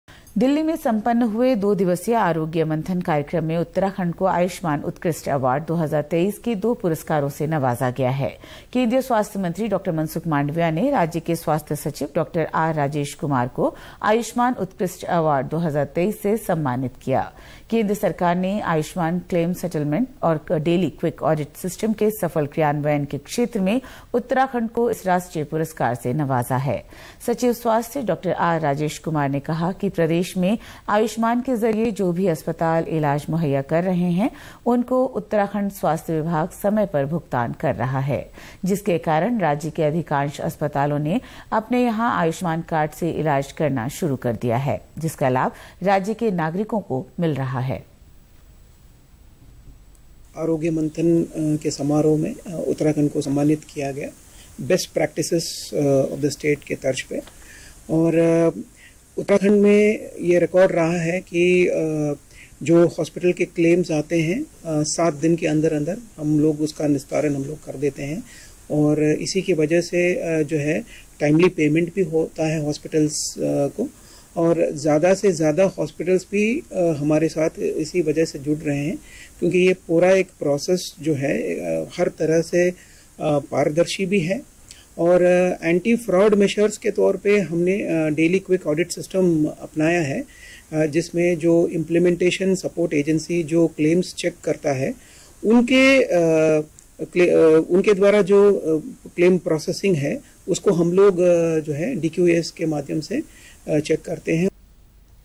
आरोग्य मंथन कार्यक्रम में उत्तराखंड को आयुष्मान उत्कृष्ट अवार्ड 2023 के दो पुरस्कारों से नवाजा गया
दिल्ली में संपन्न हुए दो दिवसीय आरोग्य मंथन कार्यक्रम में उत्तराखंड को आयुष्मान उत्कृष्ट अवार्ड 2023 के दो पुरस्कारों से नवाजा गया है। केंद्रीय स्वास्थ्य मंत्री डॉ मनसुख मांडविया ने राज्य के स्वास्थ्य सचिव डॉक्टर आर राजेश कुमार को आयुष्मान उत्कृष्ट अवार्ड 2023 से सम्मानित किया।